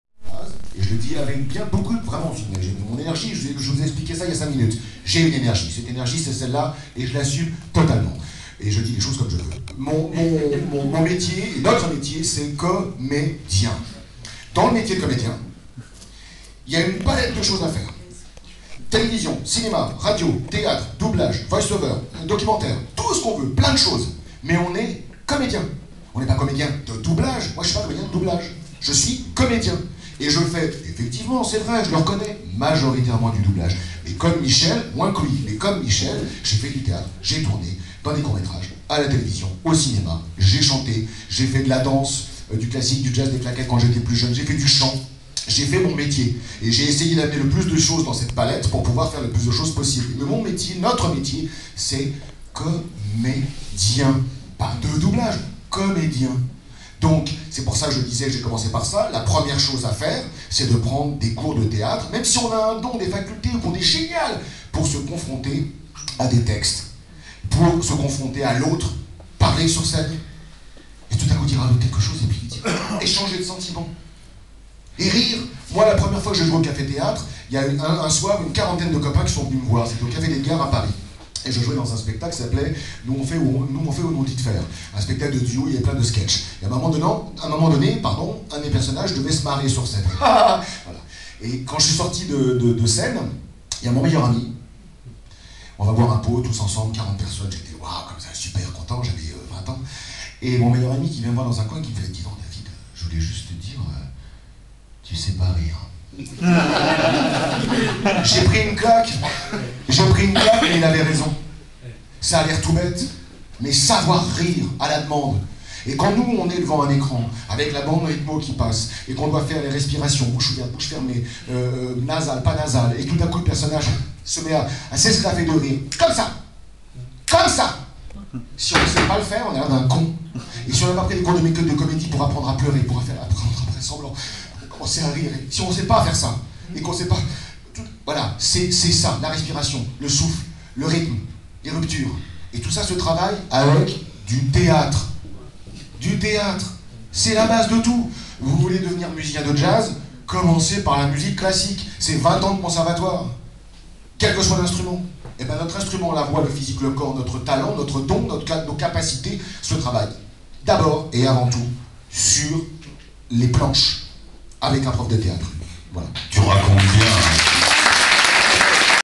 David Krüger sur le métier de comédien - Salon du doublage 2018
David Krüger explique pourquoi il n'y a pas de comédien "de doublage"